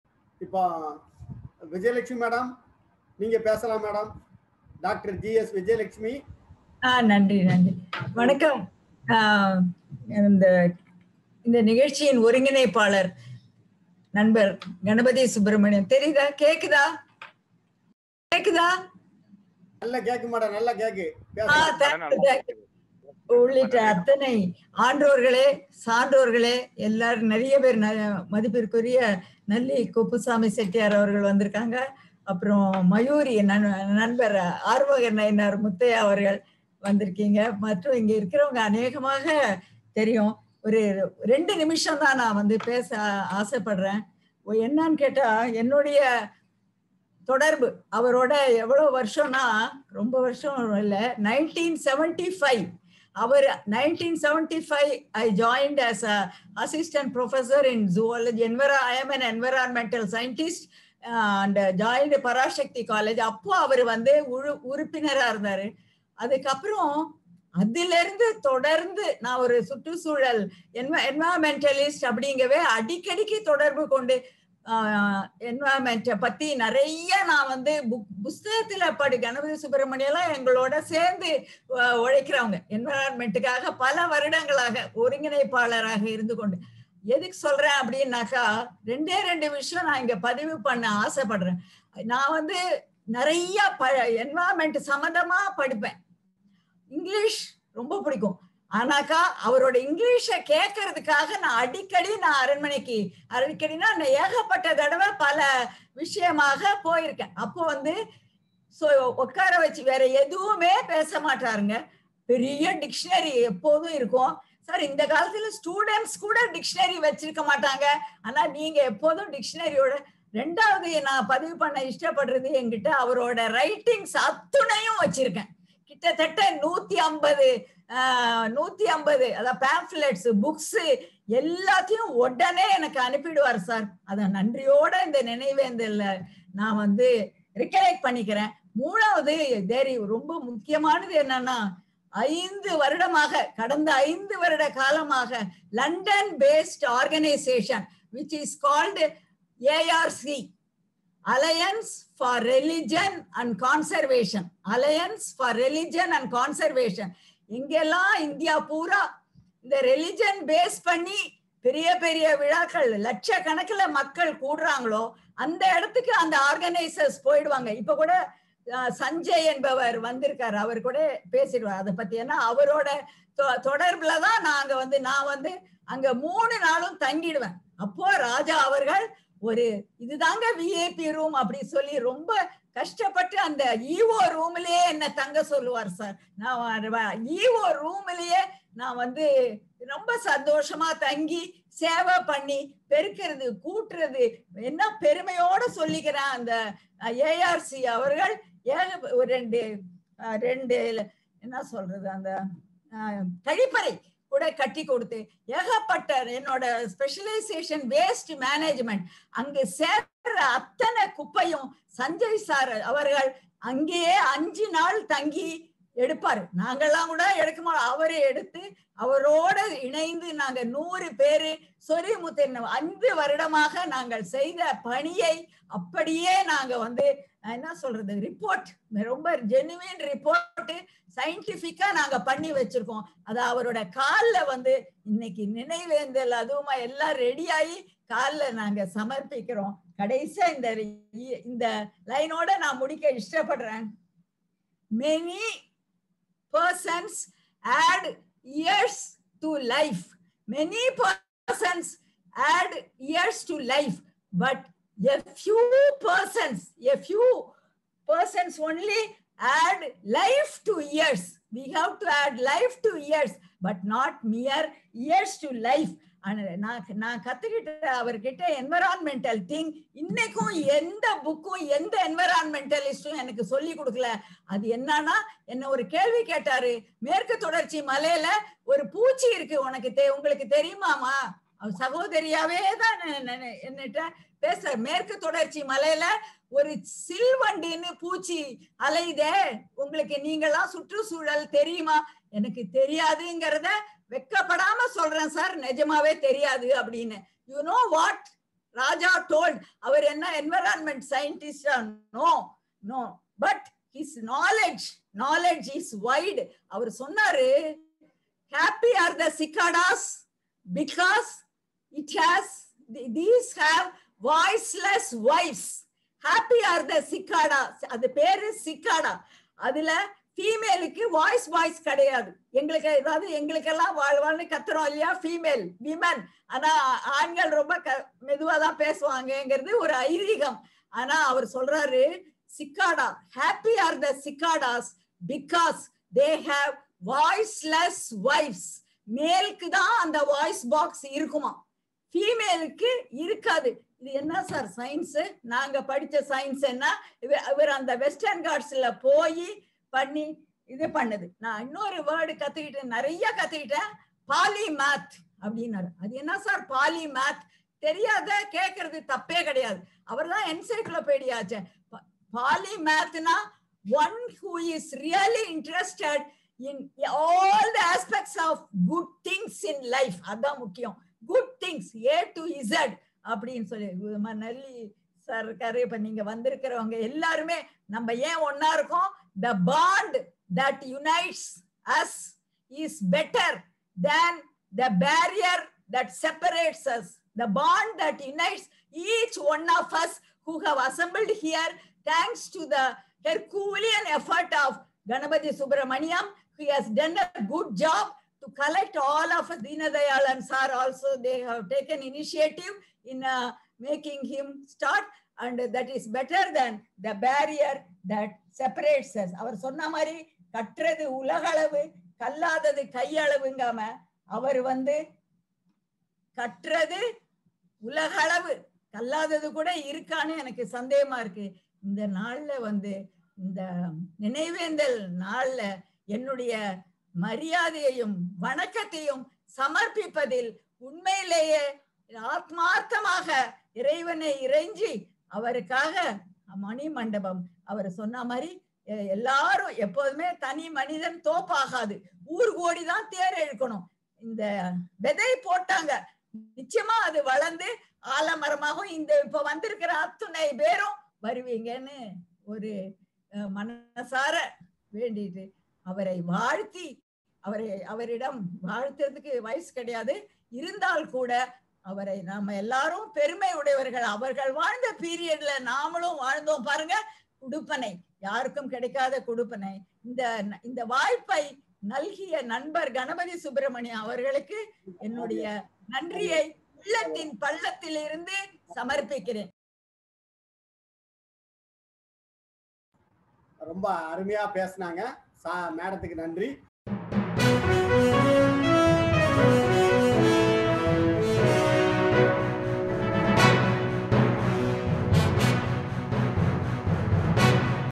சிங்கம்பட்டி TNS முருகதாஸ் தீர்த்தபதி ராஜா | முதல் ஆண்டு நினவலைகள் | THE LAST KING |dr vijayalakshmi
the last king
tns murugadass THEERTHAPATHY
first year death anniversary
chorimuthu ayyanar
forest king
 திருநெல்வேலி அரசு அருங்காட்சியகம் என்.பி.என்.கே கலை பண்பாட்டு மன்றம், சிவப்பிரகாசர் நற்பணி மன்றம் ஆகியவை இணைந்து வரலாற்று சிறப்புமிக்க சிங்கம்பட்டி ஜமீன் டி.என்.எஸ் முருகதாஸ் தீர்த்தபதி மகாராஜா முதலாமாண்டு நினைவேந்தல் இணையவழி கருத்தரங்கம் நடைபெற்றது.

 கருத்தரங்கிற்கு தமிழக அரசின் முன்னாள் அரசு செயலர் கி.தனவேல் இ.ஆ.ப. தலைமை தாங்கினார்கள் வருகை தந்தவர்களை சிவப்பிரகாசர் நற்பணி மன்ற செயலாளர் முனைவர் கவிஞர் கோ.கணபதி சுப்பிரமணியன் வரவேற்றார். திருநெல்வேலி அரசு அருங்காட்சியக காப்பாட்சியர் சிவ சக்தியவள்ளி தொடக்கவுரையாற்றினார்கள். தொடர்ந்து கலைமாமணி கவிஞர் ஏர்வாடி ° எஸ்.ராதாகிருஷ்ணன்,நல்லி குப்புசாமி செட்டியார், ராஜாவின் உறவினர் திருமதி வசந்தி ராணி, ராஜாவின் தம்பி மகன் சக்தி சட்டனநாத ராஜா, அரிமா சங்கத் தலைவர்.மயூரி.எம். ஆறுமுக நயினார் ராஜேஸ்வரன்,  பிளிப்கார்ட் நிறுவன இயக்குனர் மஞ்சுநாத், தல்லாகுளம் காவல் உதவி ஆணையர் எஸ்.சேகர், தாயுமாணசுந்தரம், எழுத்தாளர் பாளை. இசக்கி ராஜன், கடையநல்லூர் உறுப்பு கல்லூரி முதல்வர் பேராசிரியை முனைவர் வேலம்மாள் முத்தையா, சுற்றுச்சூழல் விஞ்ஞானி டாக்டர் ஜி.எஸ். விஜயலட்சுமி, ஆவின் ஆறுமுகம், விவேகானந்தர கேந்திர மாவட்டத் தலைவர் முத்துசாமி, நூலகர் அகிலன் முத்துகுமார், ரஞ்சனா நாச்சியார், ஆசிரியர் த.அருணாசலம், சோயா சரவணன் , புலவர் இளங்கோ, புலவர்பொன்னழகன் ஆசிரியர் முக்கூடல் முத்துகுமார், கவிஞர் முத்துவேல், தர்மபுரி ஆசிரியை ஹேமலதா, தைவானிலிருந்து ராமு.கிருஷ்ணன் செந்தில் மற்றும் பலர் கலந்து கொண்டனர். நிறைவாக என்.பி.என்.கே.கலை பண்பாட்டுமன்ற இயக்குநர் எழுத்தாளர் மு.வெ.ரா நன்றி கூறினார்.